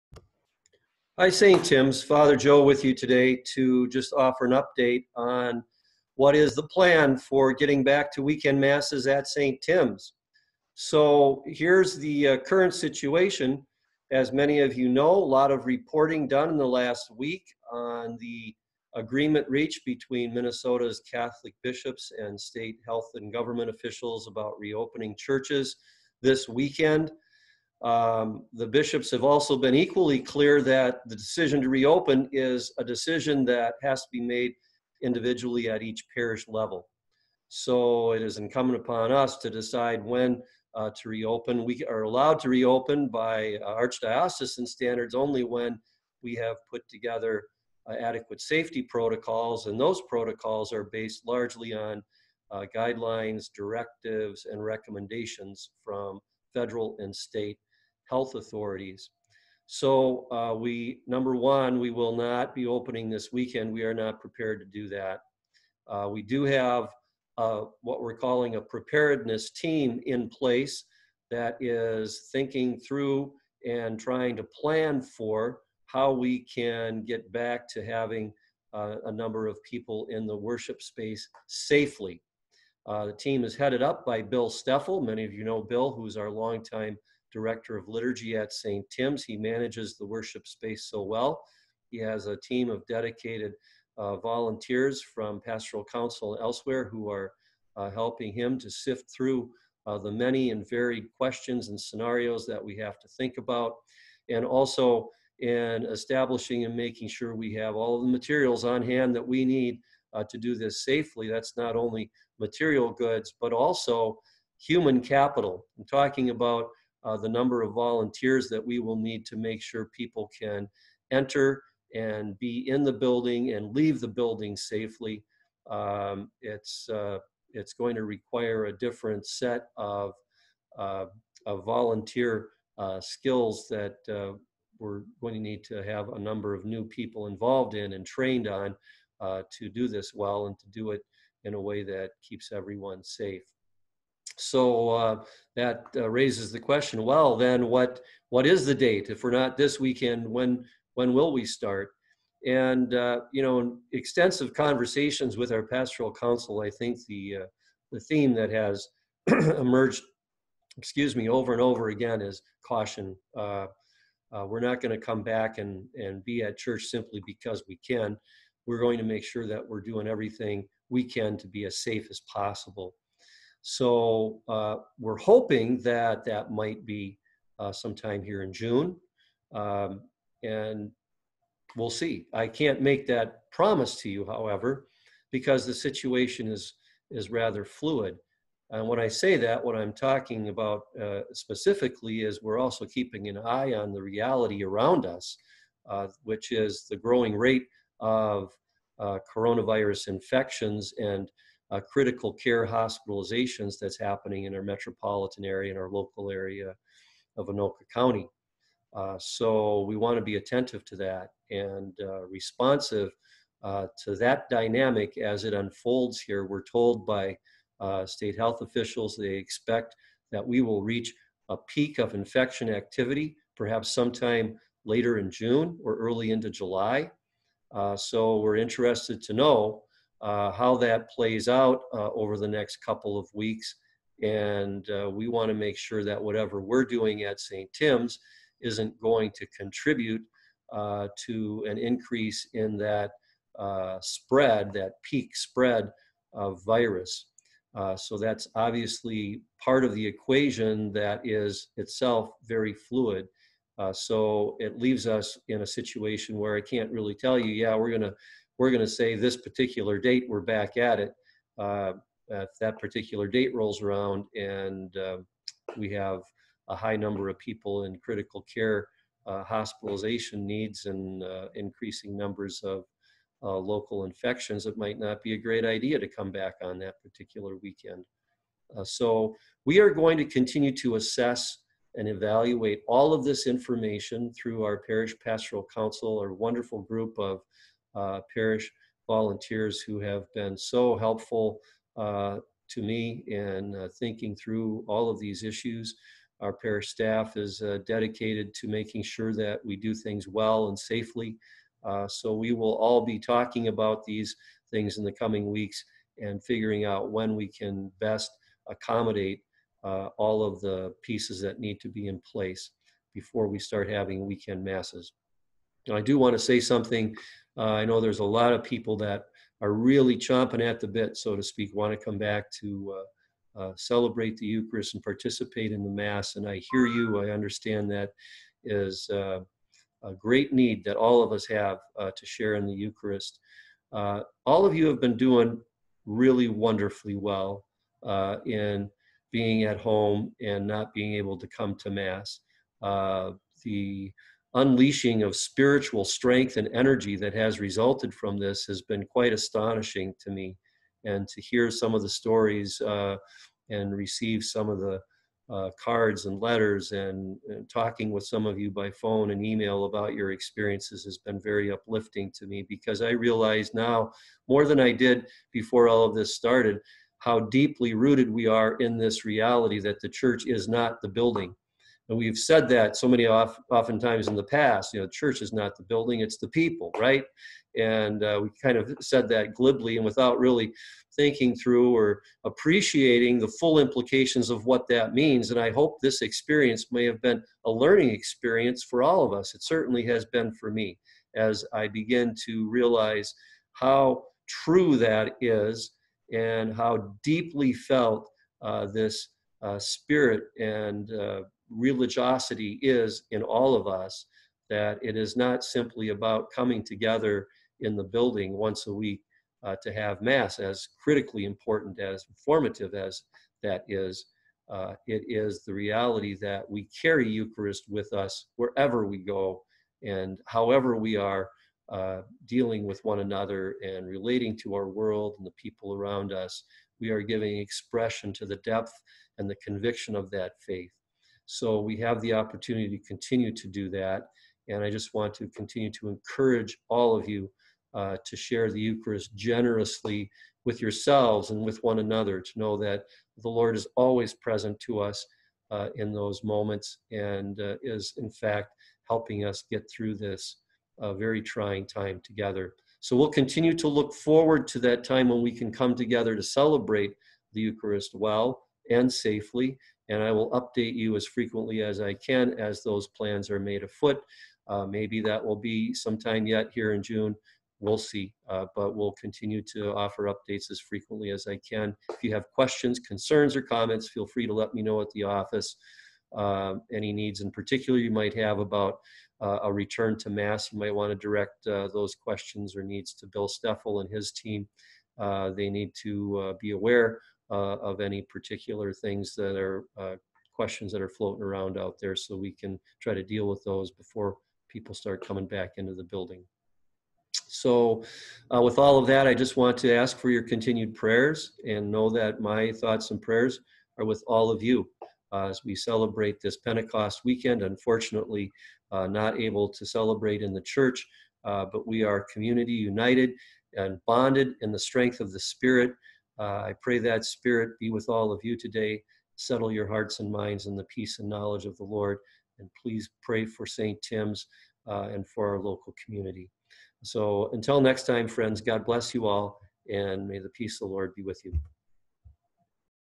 Pentecost Update
An update from Father Joe regarding the return to in-person Mass